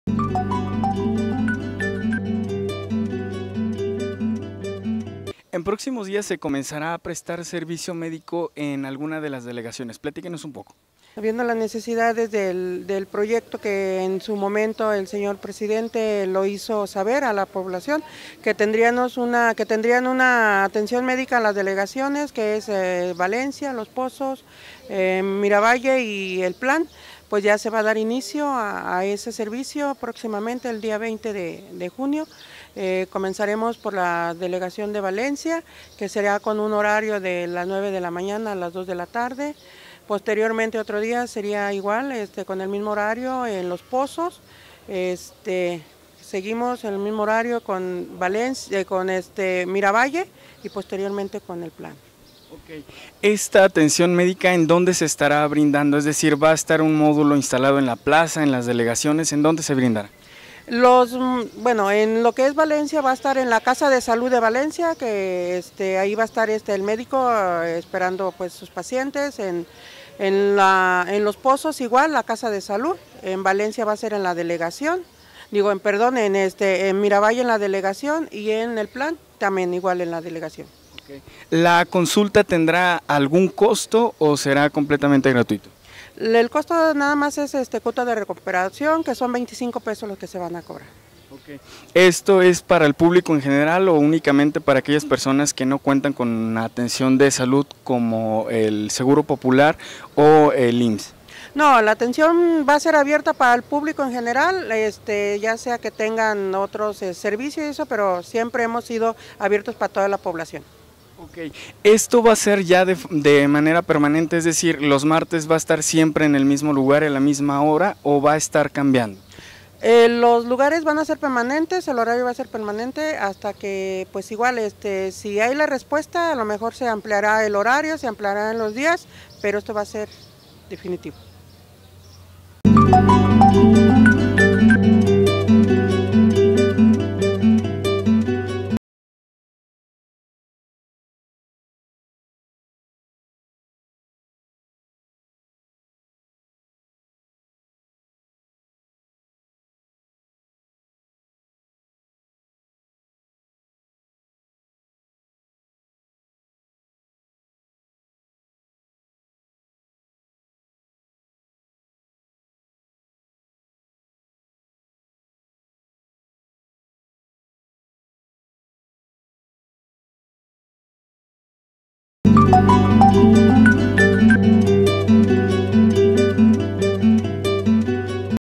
Comienza la atención médica en las diferentes delegaciones del municipio.
Gracias a las gestiones del Alcalde Gerardo Ochoa se logra conseguir atención médica en las diferentes delegaciones del municipio por medio de consultorios para atender a la población en general.